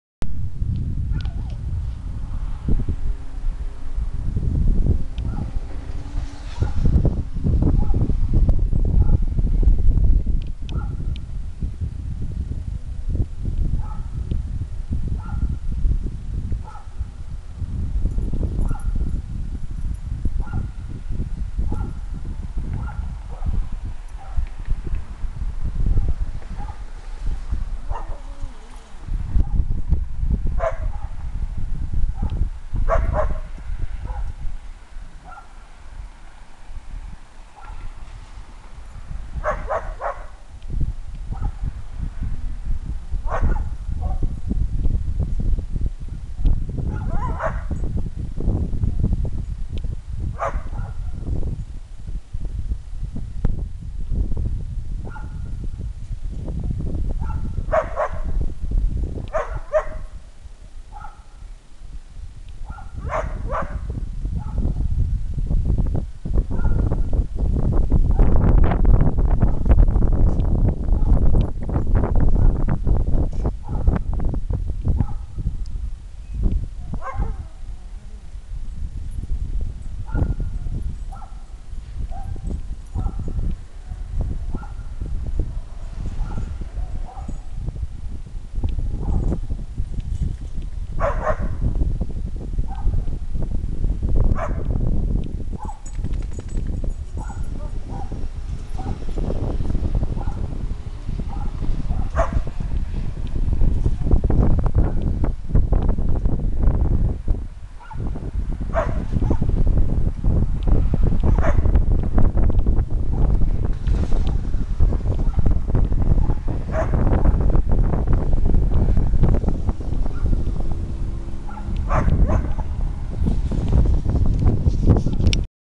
Whippet torture
Wasabi wants that squirrel so bad!!